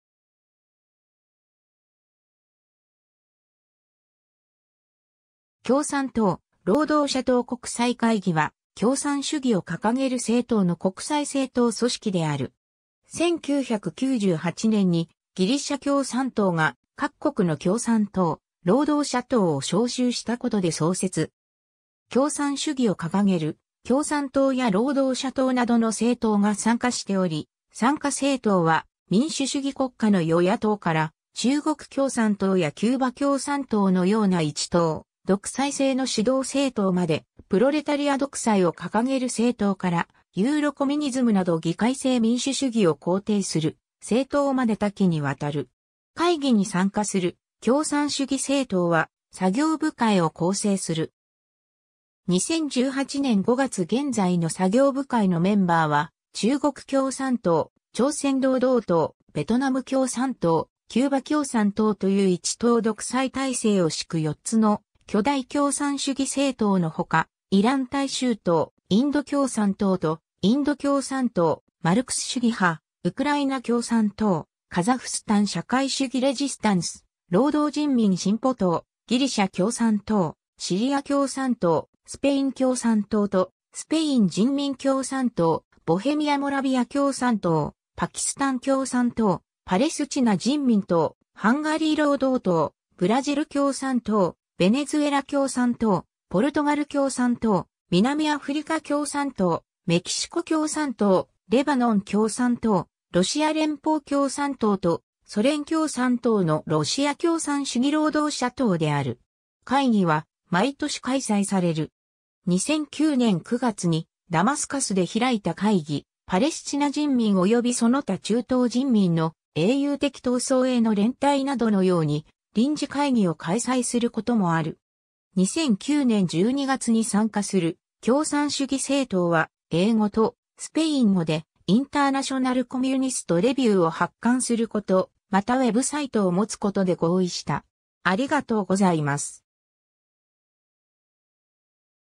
共産党・労働者党国際会議
共産党・労働者党国際会議（きょうさんとう・ろうどうしゃとうこくさいかいぎ、英語: International Meeting of Communist and Workers' Parties、IMCWP）は、共産主義を掲げる政党の国際政党組織である。
 1998年にギリシャ共産党が各国の共産党・労働者党を招集したことで創設。
共産主義を掲げる共産党や労働者党などの政党が参加しており、参加政党は民主主義国家の与野党から中国共産党やキューバ共産党のような一党独裁制の指導政党まで、プロレタリア独裁を掲げる政党からユーロコミュニズムなど議会制民主主義を肯定する政党まで多岐にわたる。
 会議に参加する共産主義政党は作業部会を構成する。
2018年5月現在の作業部会のメンバーは、中国共産党（中華人民共和国）、朝鮮労働党(北朝鮮)、ベトナム共産党（ベトナム）、キューバ共産党（キューバ）という一党独裁体制を敷く4つの巨大共産主義政党のほか、イラン大衆党（イラン）、インド共産党とインド共産党マルクス主義派（インド）、ウクライナ共産党（ウクライナ）、カザフスタン社会主義レジスタンス（カザフスタン）、労働人民進歩党（キプロス）、ギリシャ共産党（ギリシャ）、シリア共産党（シリア）、スペイン共産党とスペイン人民共産党（スペイン）、ボヘミア・モラビア共産党(チェコ)、パキスタン共産党（パキスタン）、パレスチナ人民党（パレスチナ）、ハンガリー労働党（ハンガリー）、ブラジル共産党（ブラジル）、ベネズエラ共産党（ベネズエラ）、ポルトガル共産党（ポルトガル）、南アフリカ共産党（南アフリカ）、メキシコ共産党（メキシコ）、レバノン共産党（レバノン）、ロシア連邦共産党とソ連共産党のロシア共産主義労働者党（ロシア）である。
 会議は毎年開催される。
2009年9月にダマスカスで開いた会議「パレスチナ人民およびその他中東人民の英雄的闘争への連帯」などのように臨時会議を開催することもある。
2009年12月に参加する共産主義政党は英語とスペイン語で「International Communist Review」を発刊すること、またウェブサイトを持つことで合意した